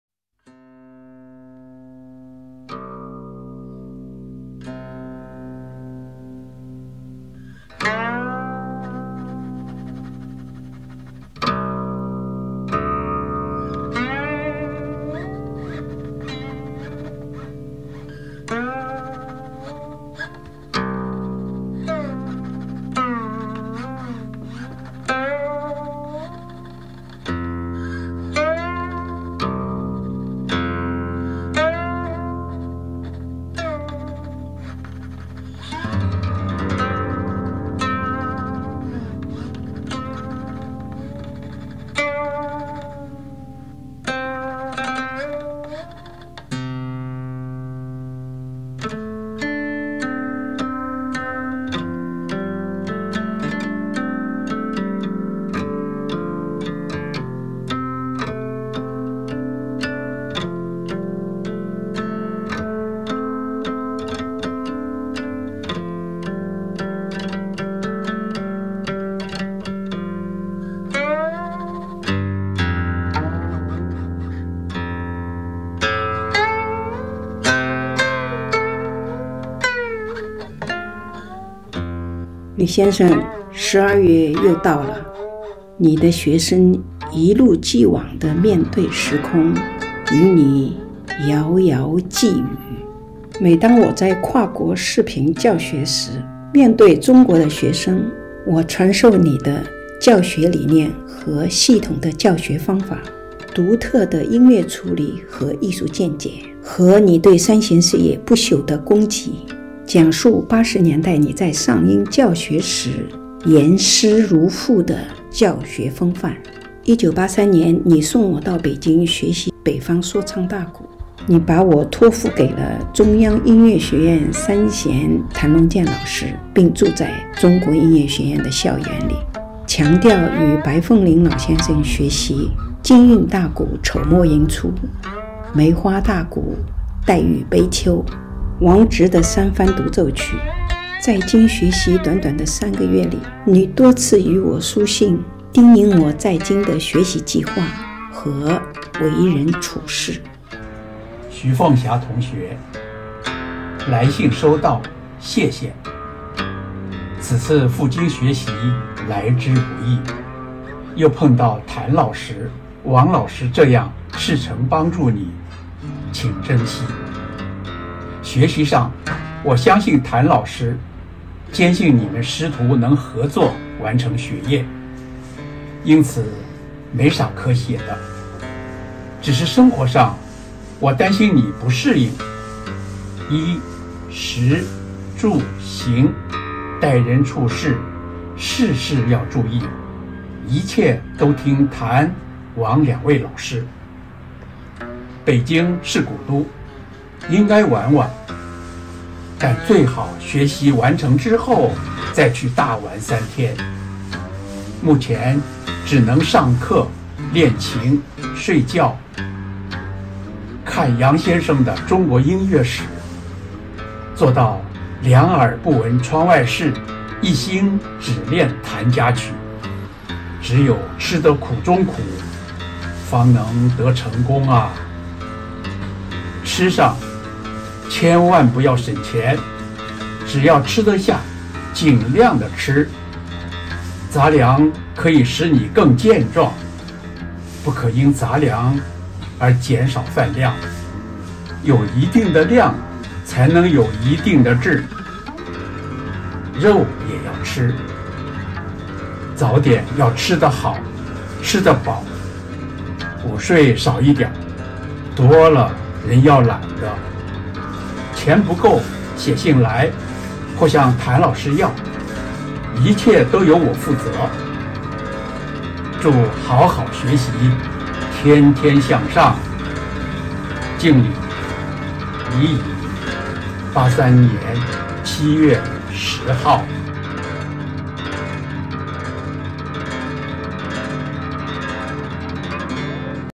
李乙先生的信 / Letter From Prof.LiYi / 1983
This film narrates the summer of 1983 when my teacher Professor Li Yi recommended me to go to Beijing to learn northern Chinese rap drums. It was a crucial inspiration for my life. In addition to caring about my studies, he also cared about all aspects of my life. It was also an important turning point in my youth. I truly understood the foundation of Sanxian art and learned northern storytelling music from the old Chinese masters.
Prof.Liyi Wrote the Letter in Shanghai Summer 1983
Guqin Xu Fengxia 1994 CD record
Prof.Wei zhongle’s version
Film Production Music Copy right
Xu Fengxia World Studio
2023.12
1983李乙先生在上海的夏天寫信與我
古琴徐鳳霞1994專輯錄音
衛仲樂先生真傳
製片音樂版權
徐鳳霞世界工作室
2023.12 德國
這部影片敘述我在1983年的夏天， 我的老師李乙教授送我到北京學習中國北方說唱大鼓，對我人生有著至關重要的啟示，在他的書信中，他除了關心我的學業，也關心我生活的各個方面，也是我年輕時代一個重要的轉折點，我真正明白三弦藝術的根基是什麼，並尋找到了中國三弦說唱音樂樂界的老前輩，